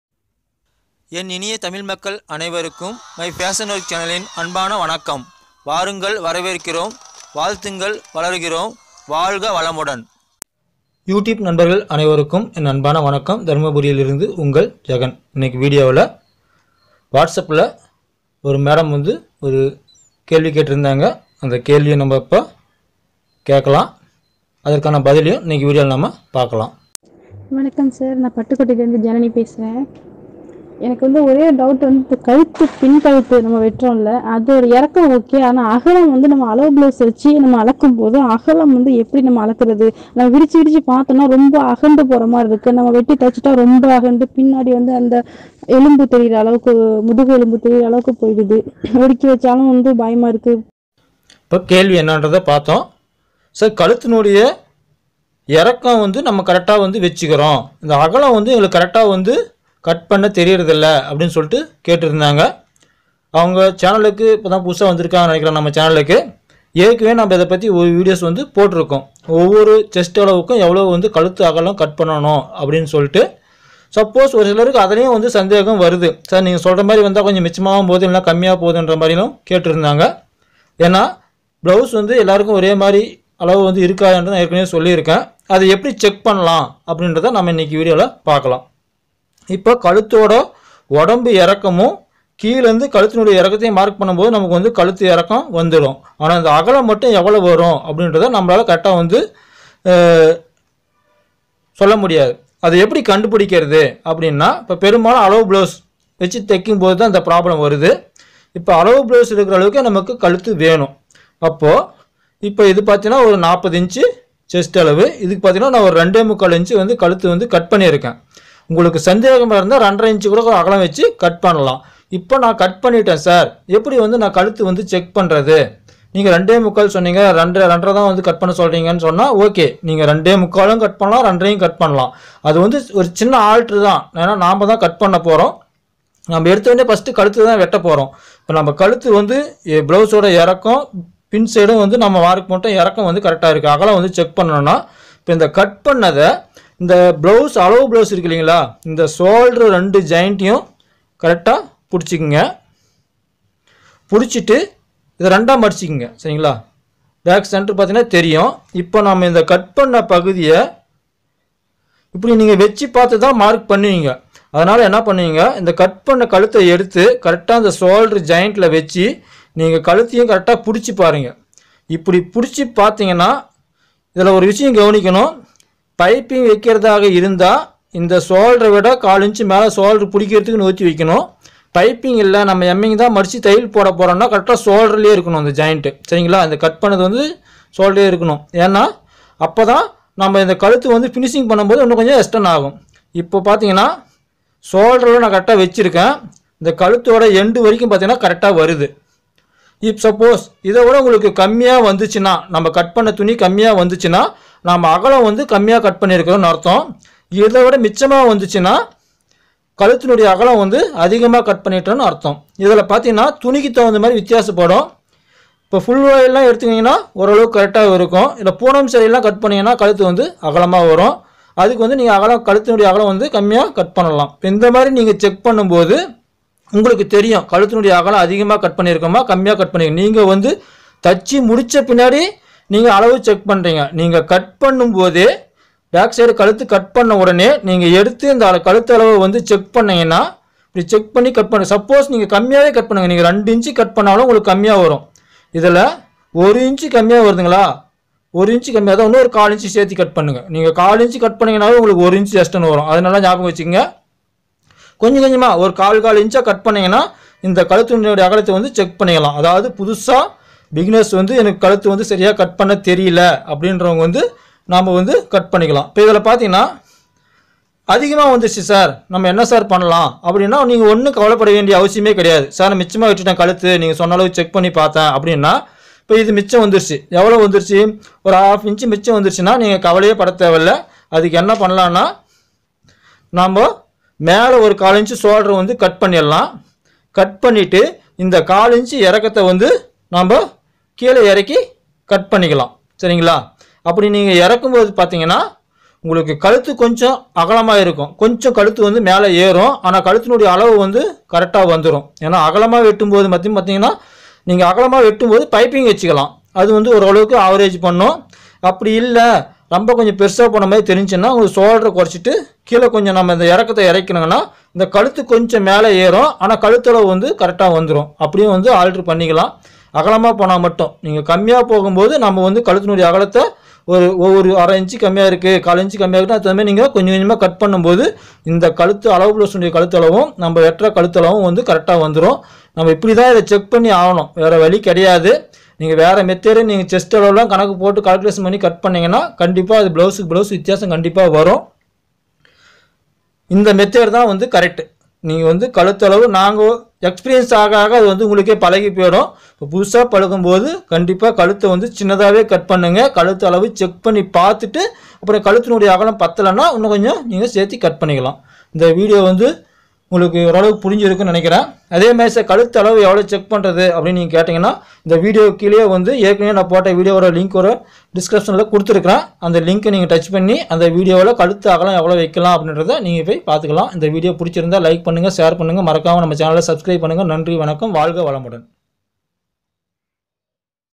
Blouse tutorial question and answer
பிளவுஸ் கழுத்து அளவு எப்படி சரியாக வெட்டுவது என்று வாட்ஸ் அப்பில் கேட்கப்பட்ட கேள்விக்கான பதில் இந்த வீடியோவில் பதிவு செய்து உள்ளேன்